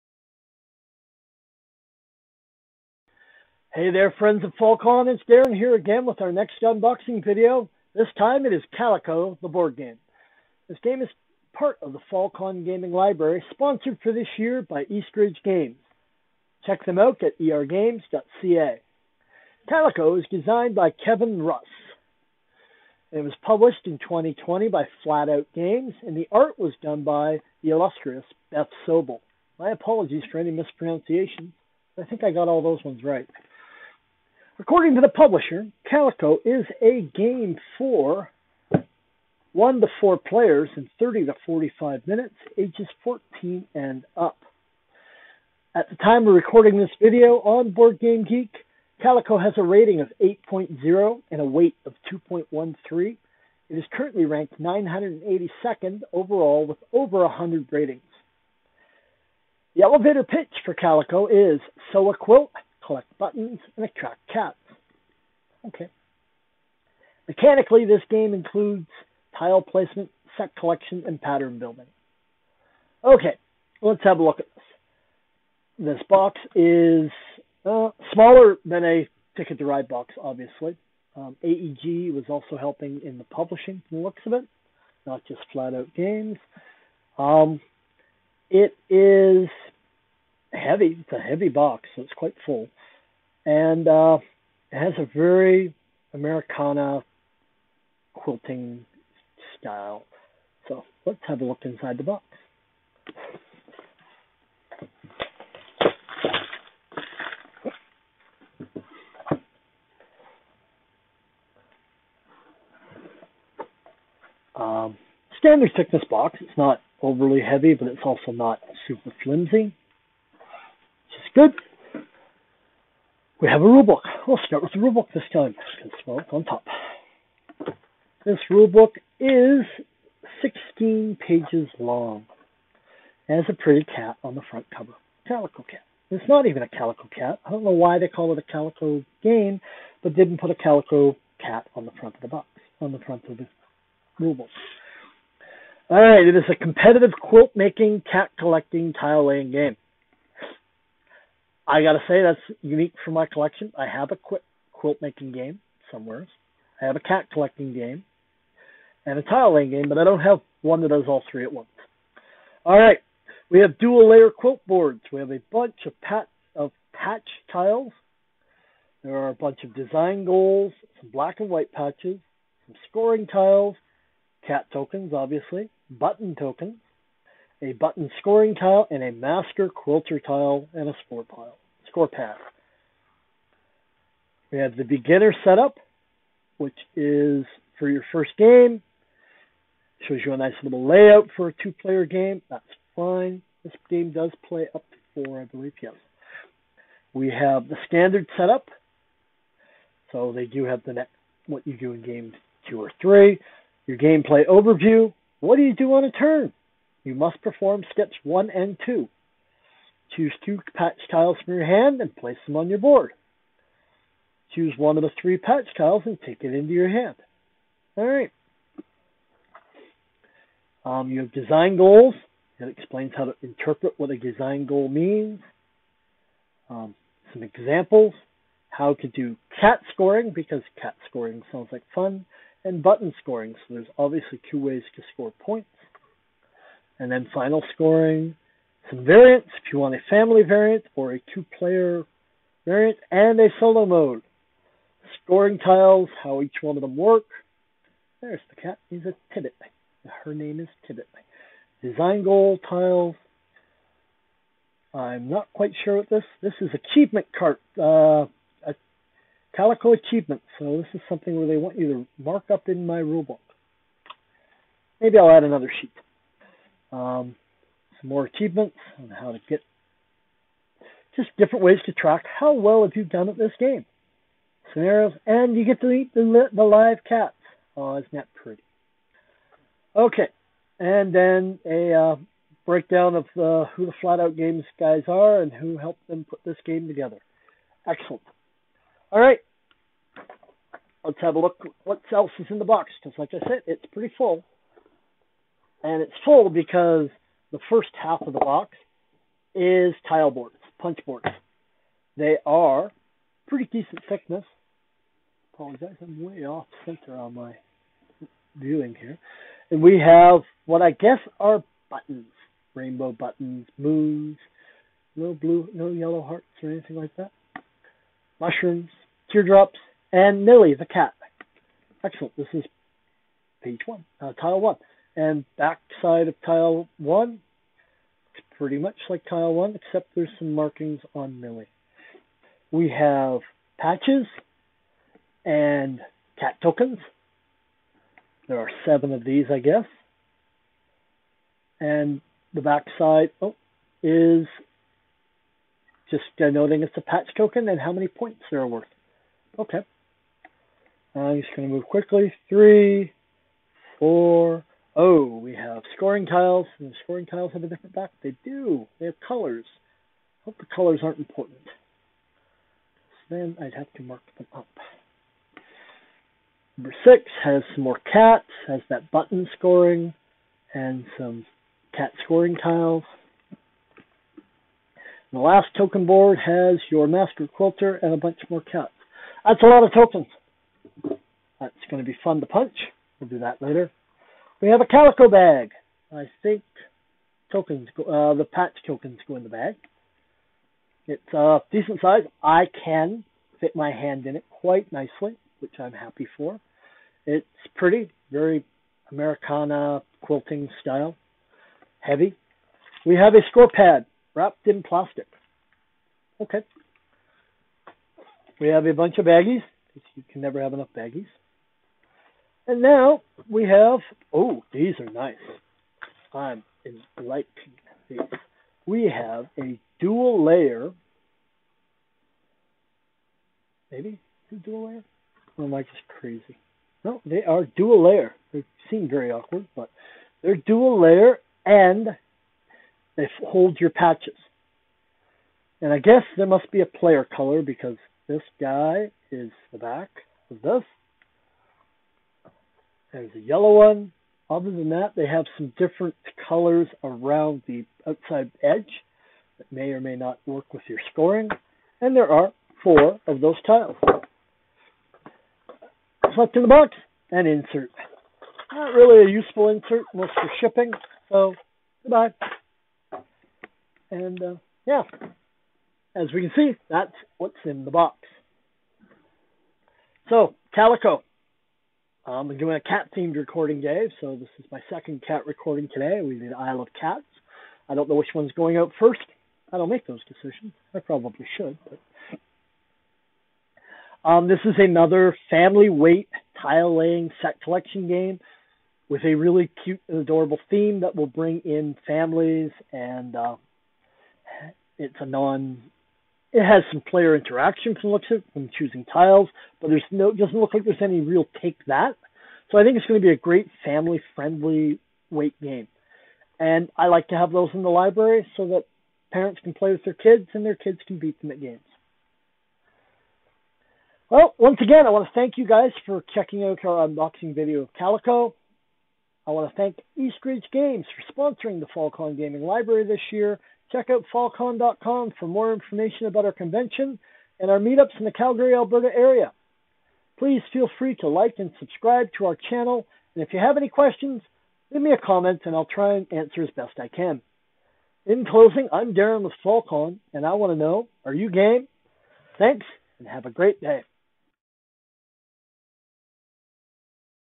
Fallcon Presents: Calico
FallCon Gaming Society presents Unboxing Calico.  This game was designed by Kevin Russ, published by Flatout Games and features art by Beth Sobel.